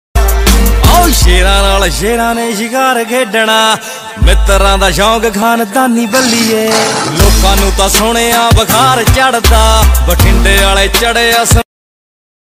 duck hunting in Pakistan#tips for duck hunting#murghabi ka shikar tarbela jheel kpk main#
set up for our duck(murghabi) hunting🥰